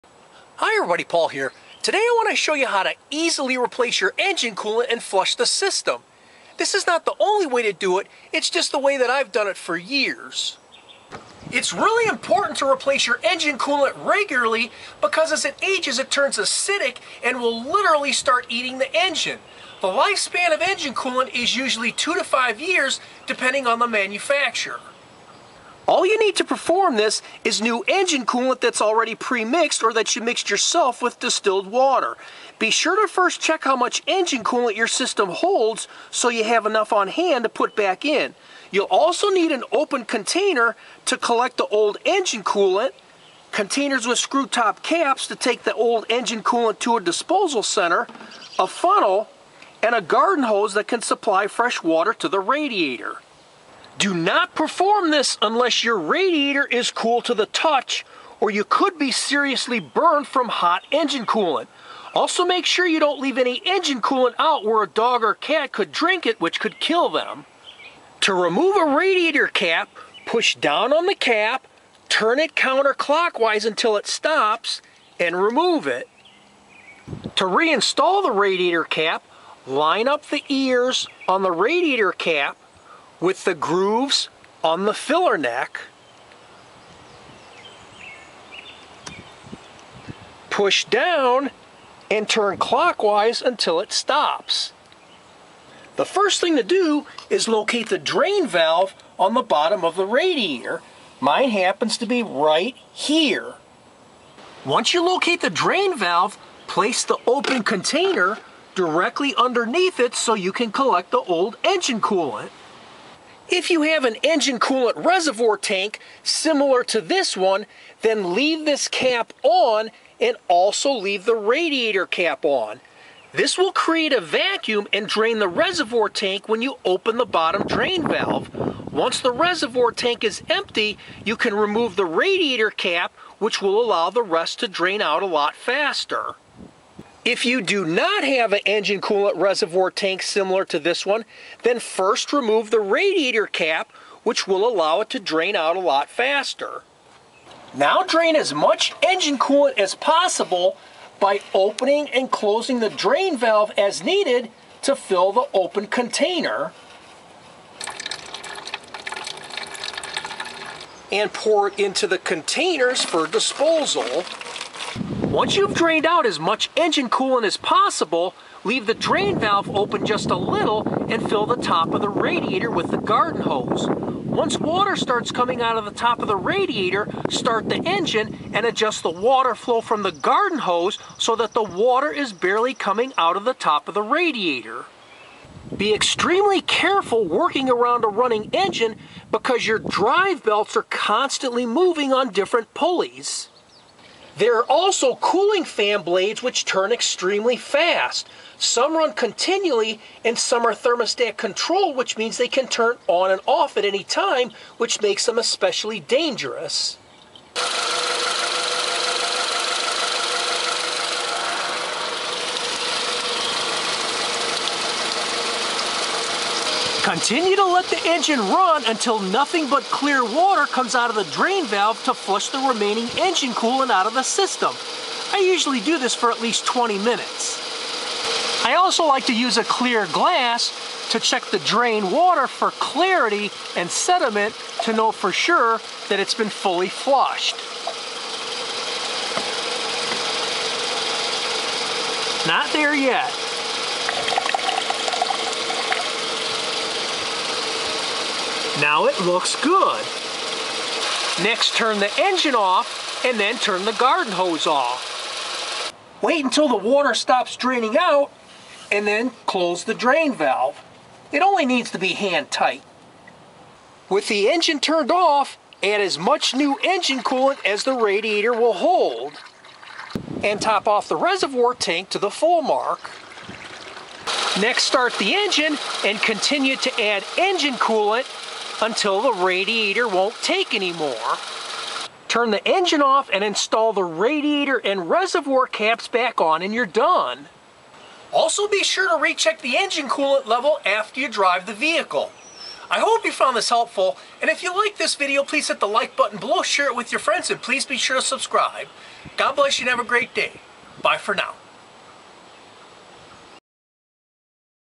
NEVER Replace Your Engine Coolant Until Watching This!
Watch at proclaimliberty2000 how to SUPER FLUSH your vehicles cooling system without costly equipment or splicing into cooling hoses.